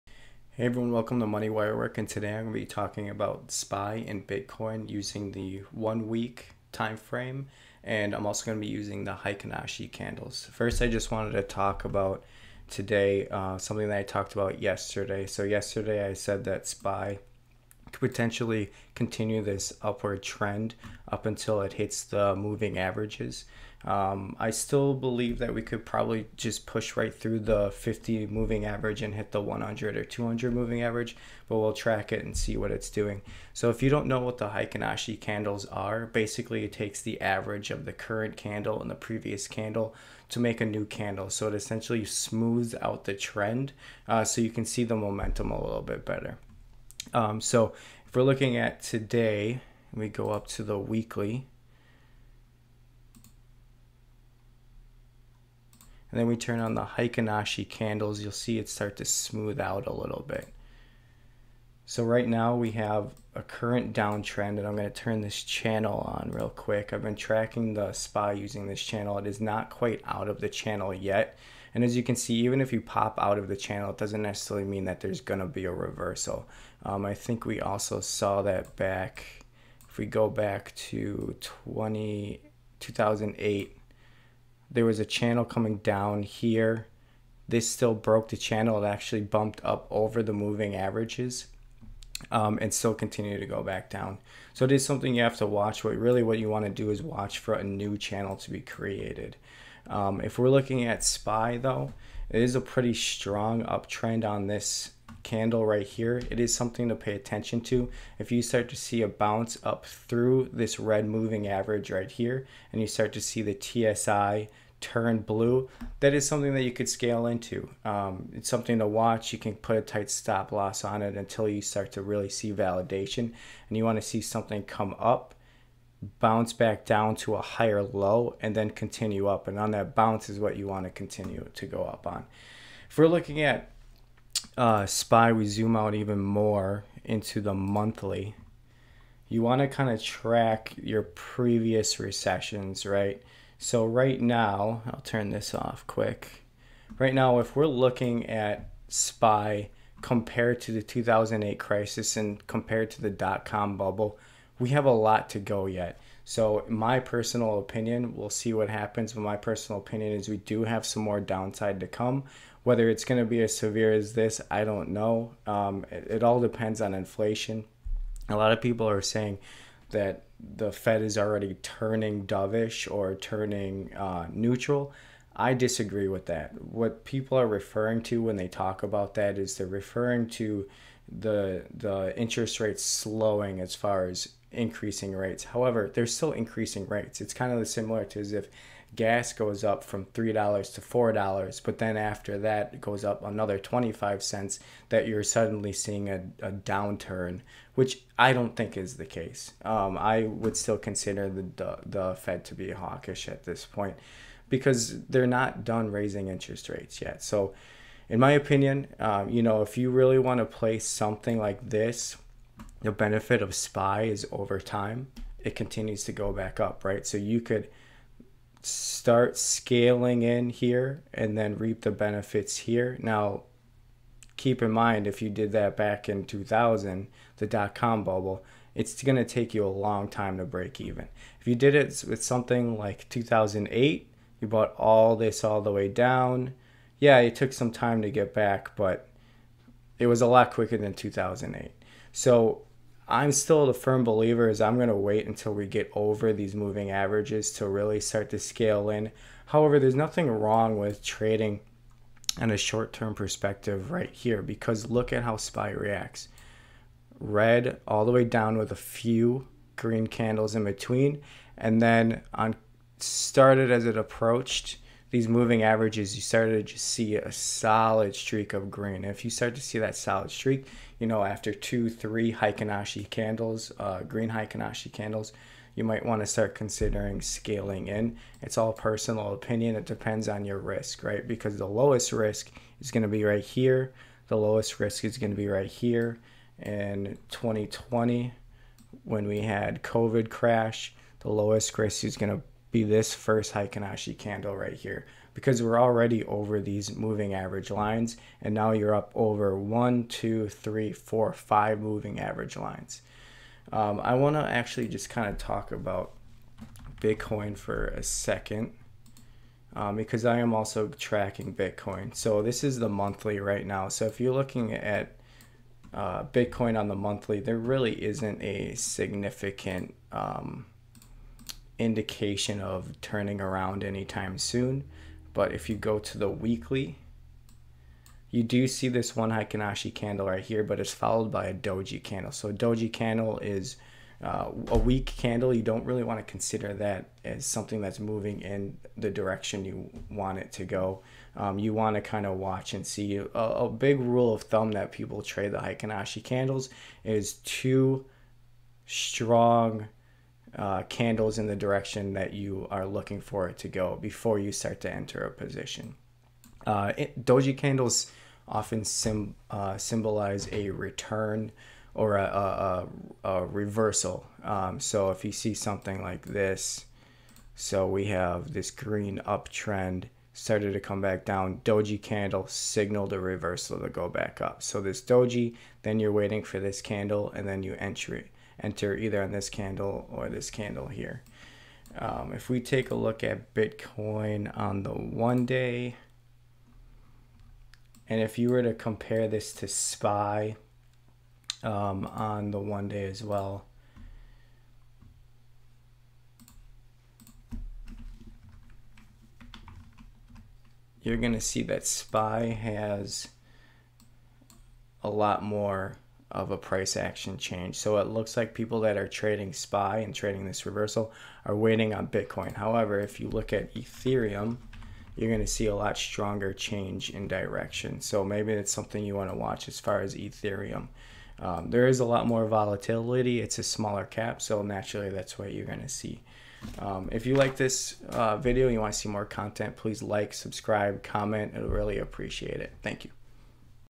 Bitcoin and SPY using the Heiken-Ashi Candles
In this video I go over Bitcoin and SPY using Heiken-Ashi Candles.